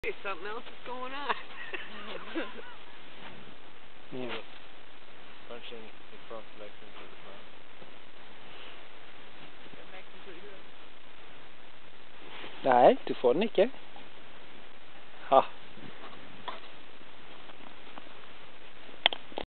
A wolf plotting to steal my glove
This socialized wolf is trying to steal my left glove while I'm filming her.